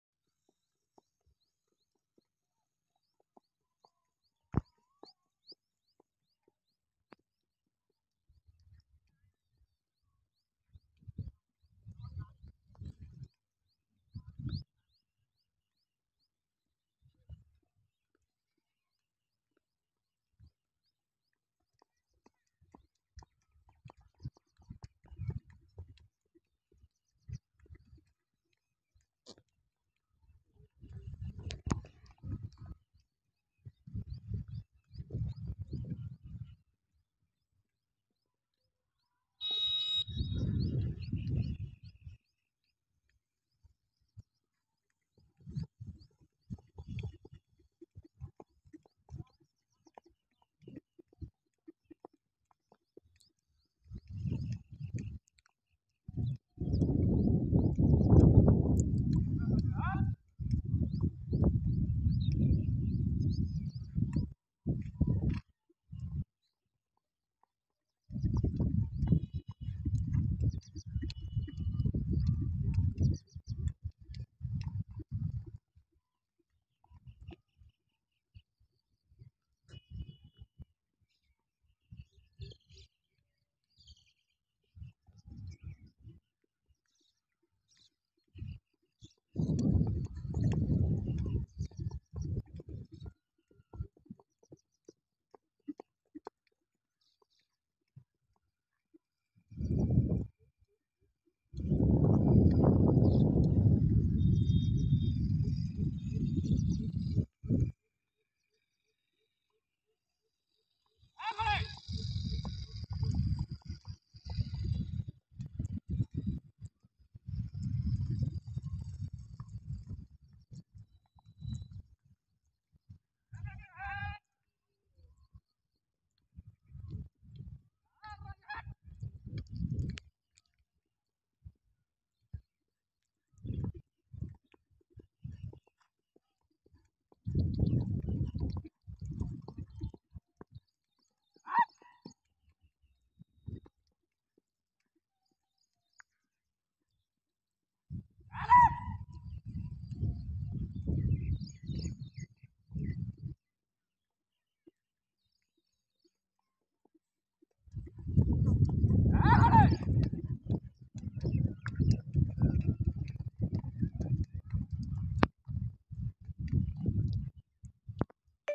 बहुत ही शानदार घोड़े की तैयारी का वीडियो आप सभी देख रहे हैं इस वीडियो में
आप देख रहे हैं इस वीडियो में एक घोड़े का दौड़ वीडियो